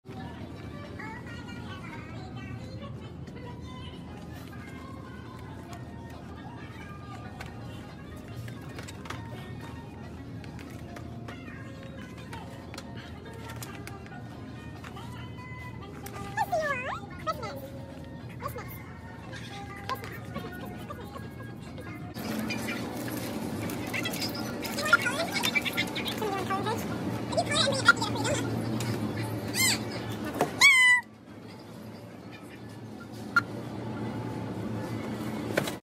TRUMAN got WHAT chocolate @GeneralMills cereal??’
Since TRUMAN didn’t find chocolate @chex cereal yesterday, he found a chocolate chex cerealbox at a different @MaceysGrocery in American Fork, UTAH on 12-19-23

Chocolate Chex Cereal